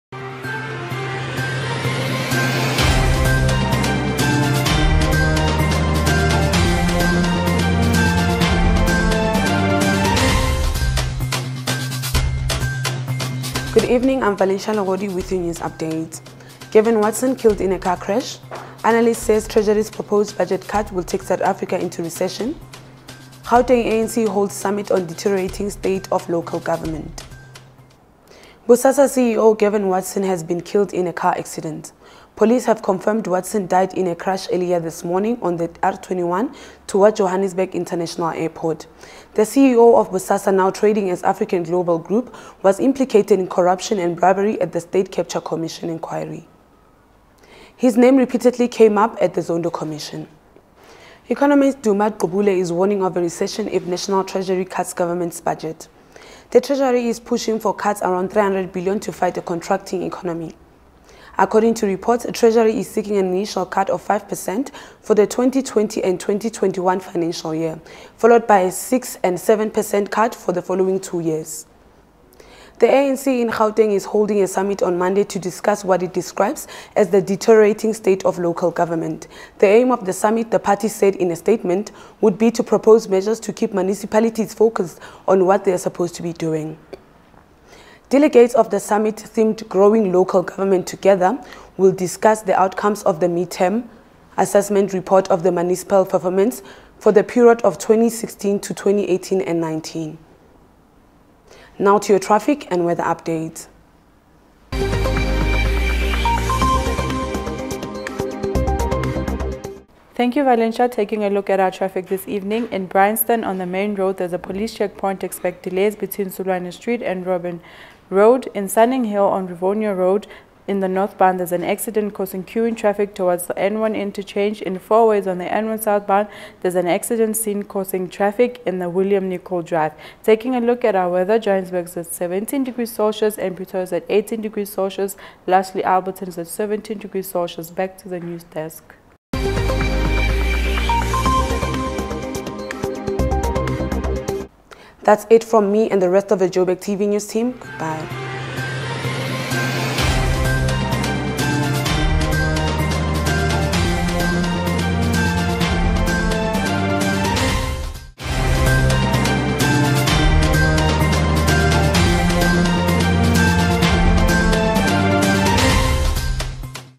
Evening Bulletin - Joburgtv News - 26 August 2019
ANC mourns Gavin Watson's death.
The African National Congress (ANC) has reacted with shock to the death of African Global Operations (formally Bosasa) CEO Gavin Watson.
In a statement, the ANC described Watson as a dedicated anti-apartheid activist.

"Cde Gavin in particular also played a significant role in providing support, through the family businesses to many activists and operatives of uMkhonto weSizwe who were being pursued by the police and fleeing Port Elizabeth and the Eastern Cape as a whole,  at the time.

“He extended this role by mobilizing other white business persons to support and join the effort of creating a democratic and free South Africa for all."
Watson died in a crash while driving a company car at OR Tambo International airport in the early hours of Monday morning.

Police have opened a culpable homicide case while his family has released a statement requesting privacy.

He was expected to testify at the commission of inquiry into state capture following testimony by his former COO Angelo Agrizzi, implicating him in corruption worth billions of rands.